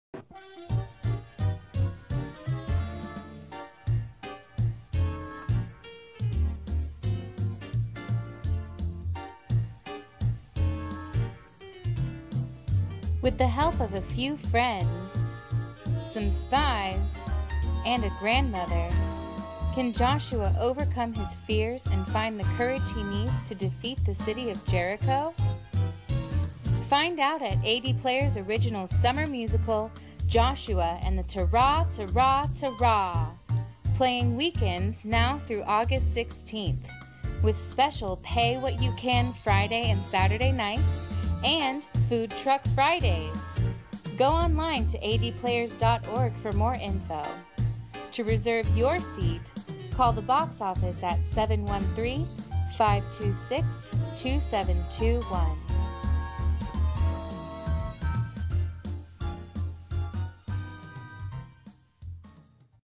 Joshua and the Ta-Ra Ta-Raa Ta-Raaa! TRAILER
This Summer! Joshua and the Ta-Ra, Ta-Raa, Ta-Raaa! Playing at A.D. Players Theater July 14- August 16.

Featuring Food Truck Fridays and "Pay-What-You-Can" Friday and Saturday Nights!